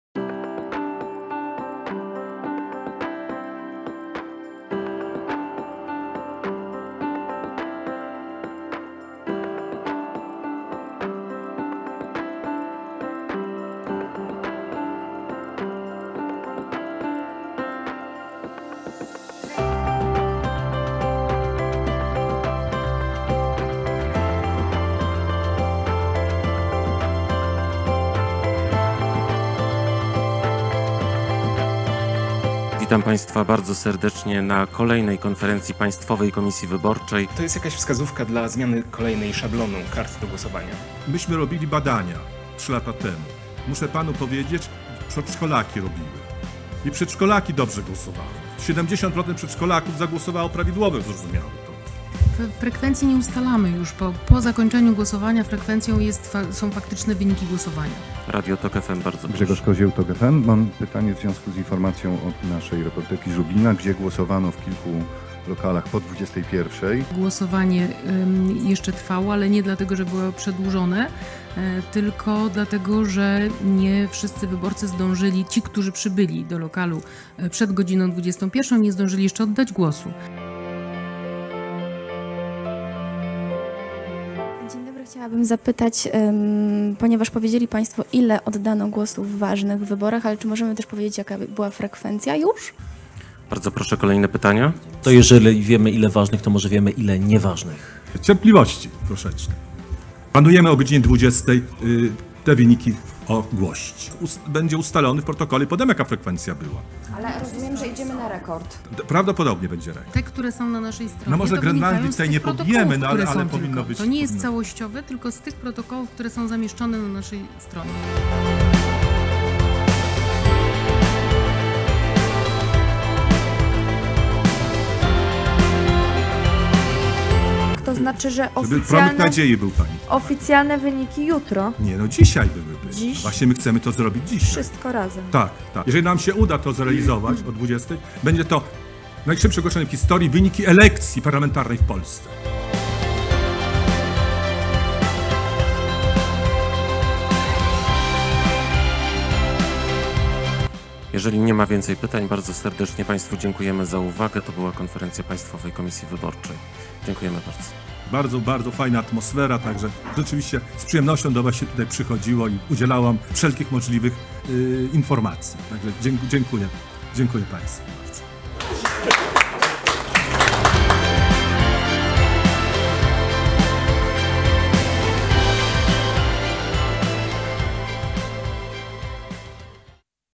PKW rekordowo szybko podała wyniki wyborów do Sejmu i Senatu [MAKING OF]
PKW ogłosiła wyniki wyborów do Sejmu i Senatu w niespełna dzień od zakończenia głosowania. Zajrzyj za kulisy! Zapraszamy do obejrzenia materiału MAKING OF.